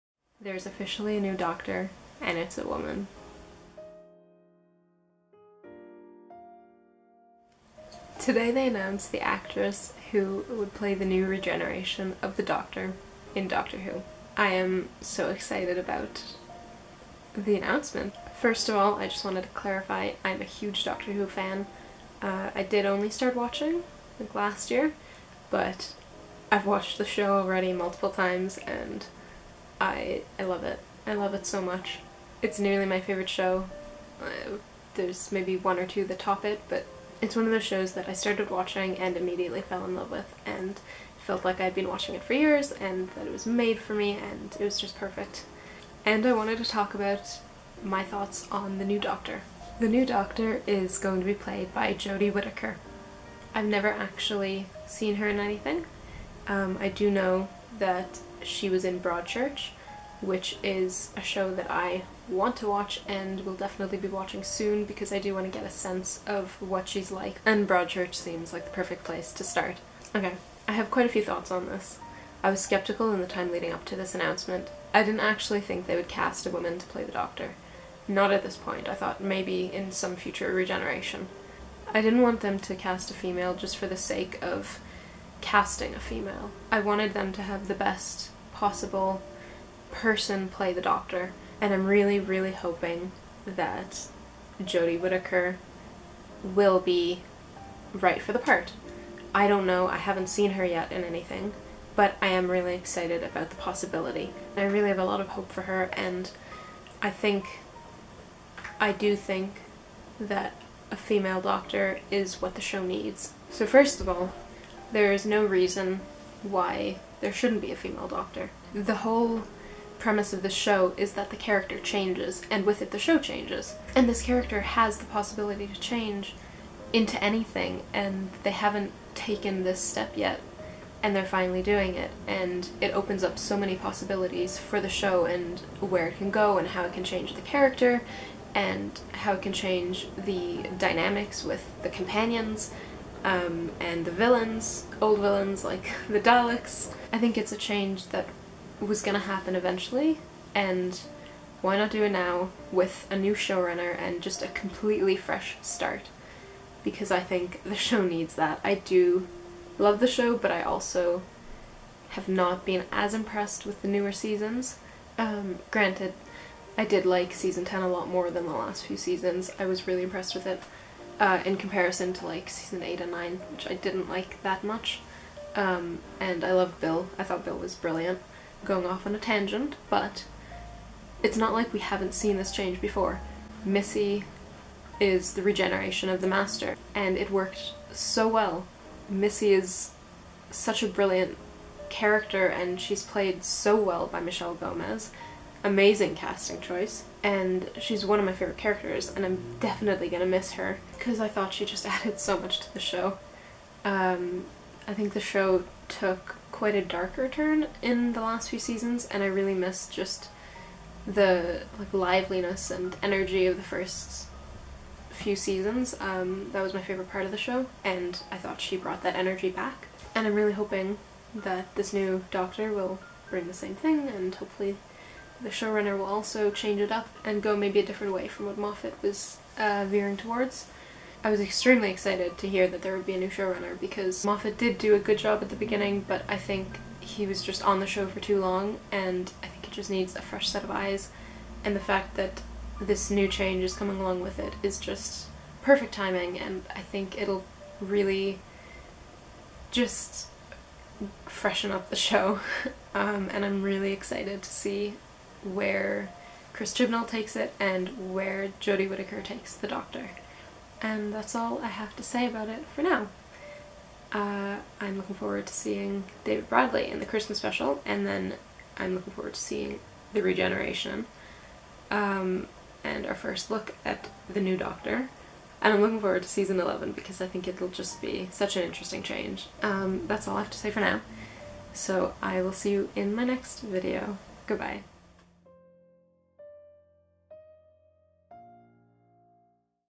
13th Doctor Discussion - Doctor Who | 2017
Here are my thoughts on the new doctor! I know there's been so much controversy, even in the first day after the announcement, but I am personally very excited and think that Jodie Whittaker just needs to be given a fair chance before any judgement is made.

I'd love to hear your thoughts on the topic, and perhaps start a conversation, but I do ask that anything commented is kept civil and kind.